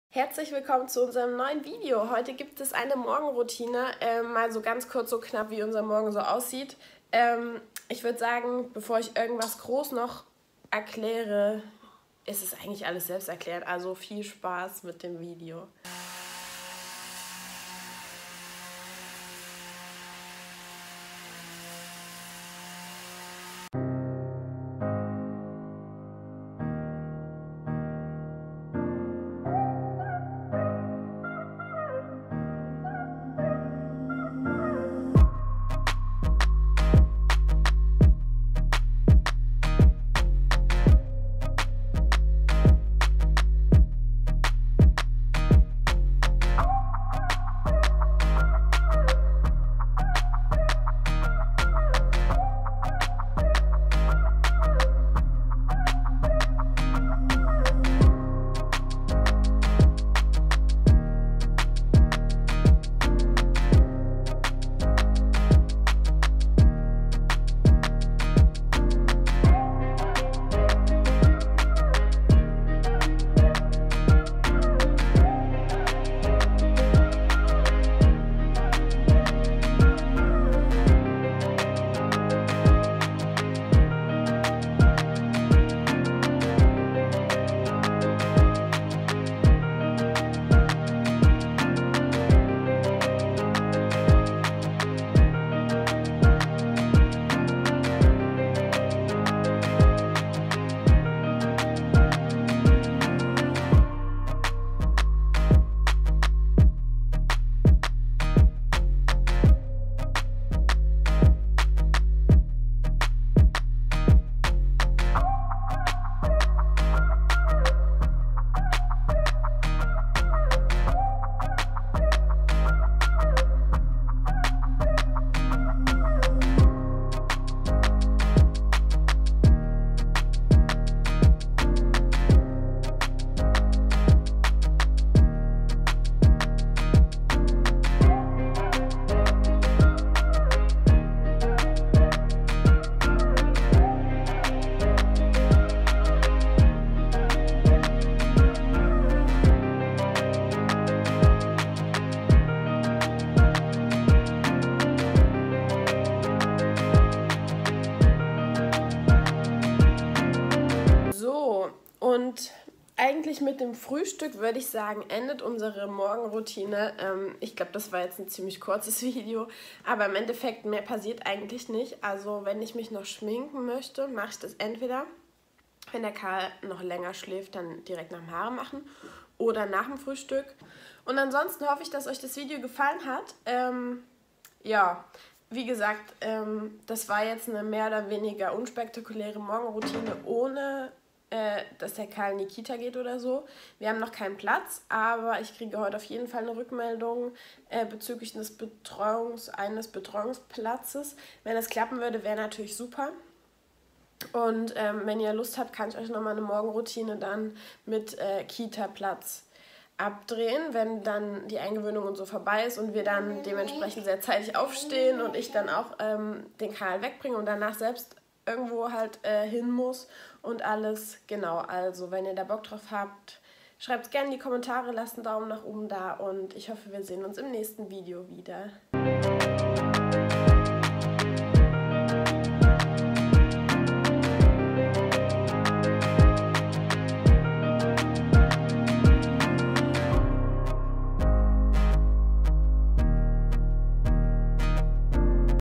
Unsere Morgenroutine | Noreia
Heute nehme ich euch mal mit an einem typischen Morgen, und zeige euch unsere Morgen-Routine. Viel Spaß mit dem Video!